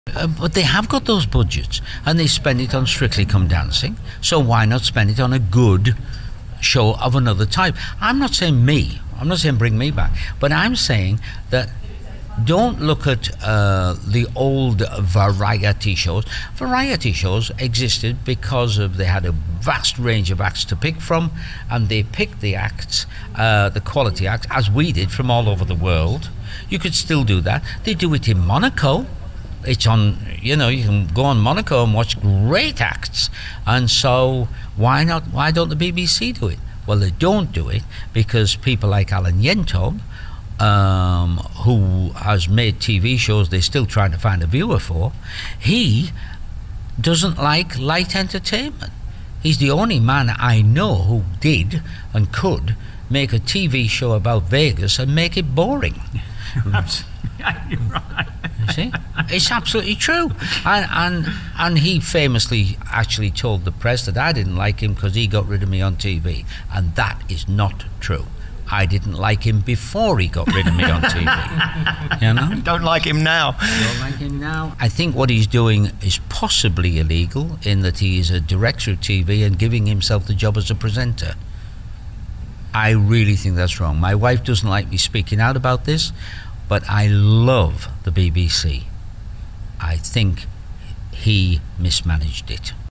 Paul Daniels Interview - "Alan Yentob Illegal & Mismanaged The BBC"
What was supposed to be a nice quiet chat turned into an explosive interview about the BBC, its management & Operation Yewtree.

Paul Daniels & Debbie McGee are two of the most famous couples in showbiz, let alone magic.

Since the 80's Paul has been one of the most respected magicians in the WORLD. From Lance Burton to Copperfield & Mac King in Las Vegas - they all rate him as the absolute best.

Paul & Debbie shot to fame on the BBC in the 80's with their Primetime Saturday night magic show celebrating the biggest illusions, Paul's unique wit and Debbie's glamour. "Say 'Yes' Paul!"

Alex Belfield went to meet Paul & Debbie backstage for a rare, in-depth and Exclusive 30 minute interview about their remarkably successful life and career.

Hear about Paul's tenacity, rise to fame and opinions about the BBC. Paul describes how you could become a Master Magician and why Street Magic is "ugly!". Debbie also tells Alex why she went weak at the knees when she first met Paul McCartney. who knew who she was!

Debbie presents a weeks Sunday show @ BBC Radio Berkshire & Paul & Debbie are on tour throughout 2014 @

Paul Daniels - The First Farewell Tour comes to the Jermyn Street Theatre in the West End at the end of Feb 2014.

In part 1 of this rare and Exclusive interview, Paul tells Alex about his life in Showbiz and how he was one of the first to put the business into show. Debbie also describes the terrible floods that surrounded their reported £2.5 mansion - now that's magic!

Enjoy a rare, in-depth and Exclusive 30 minute interview with Paul & Debbie with Alex Belfield @